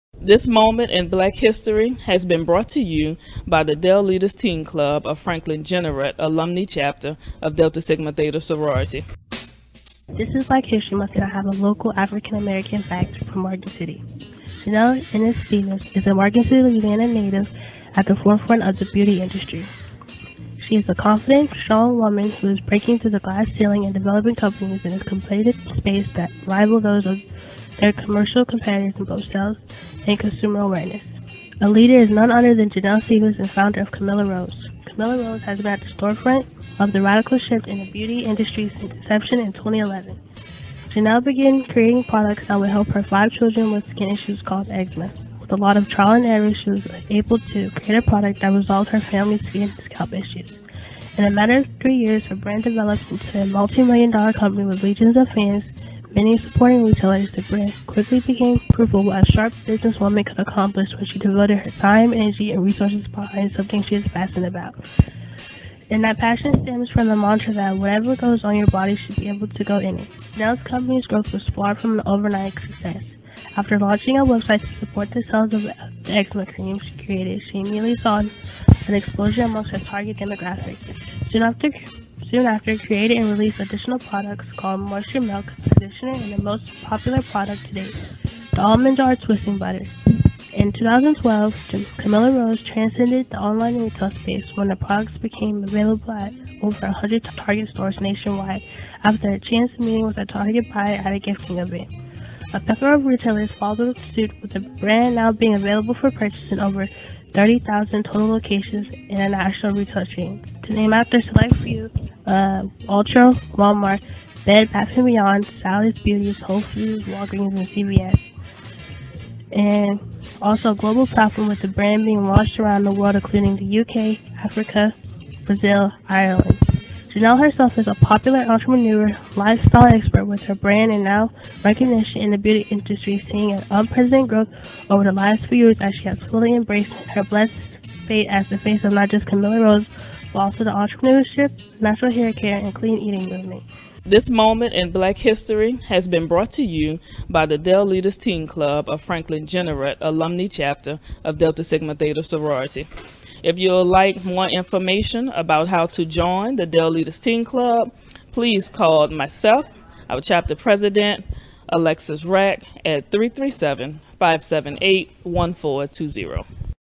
Del Leaders Club Black History Moment #16, Morgan City native Janell Stephens, camillerose.com
First air on KBZE 105.9FM - Feb 26,  2024 -
An outreach initiative by The Del-Leaders Club of the Franklin-Jeanerette Chapter of Delta Sigma Theta Sorority, Franklin, La., salute Black History 2024.